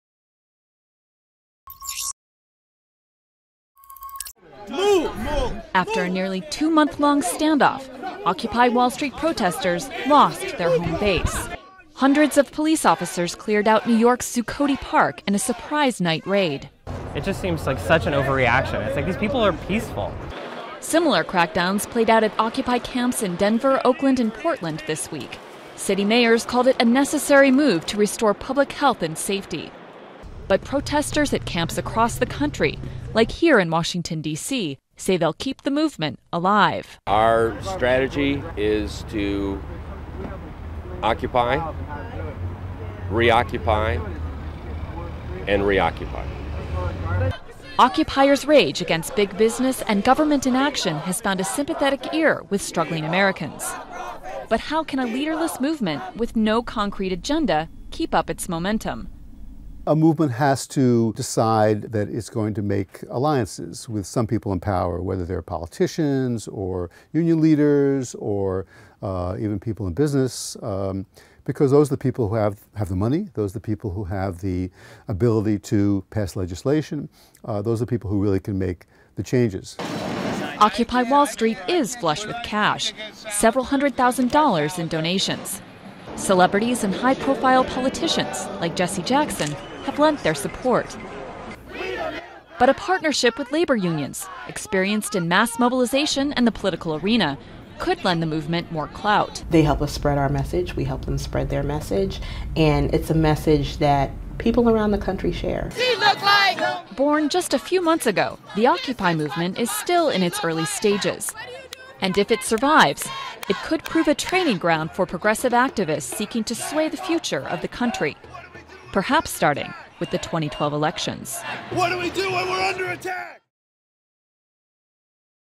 'Occupy' protesters vow to continue movement
UNITED STATES, New York : US authorities may have closed the New York camp that started the whole Occupy Wall Street movement -- but protesters say their fight against economic injustice can't be so easily shut down.